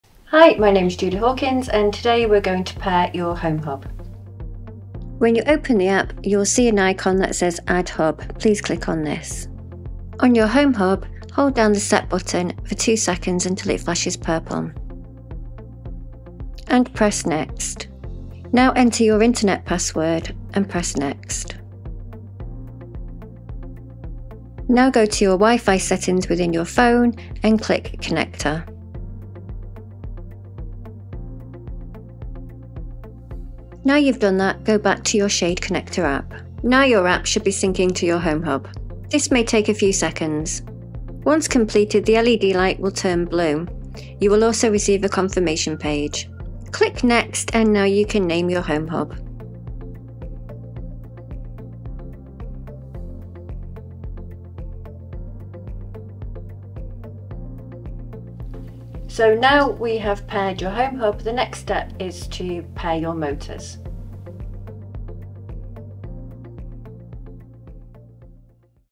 Ellard Blinds: How to pair your Home Hub to the Shade Connector app
In this video, we learn how to pair your Home Hub System to the Sade Connector App.